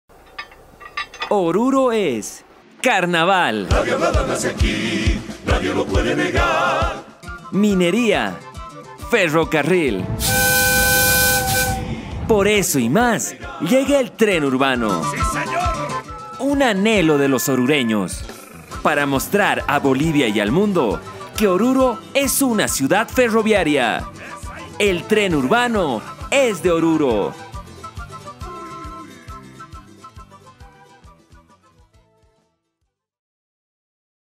"QAMAQI" ¡El tren urbano es de Oruro!
El tren Urbano es una realidad, gracias a un esfuerzo colectivo, queremos contribuir al desarrollo de Oruro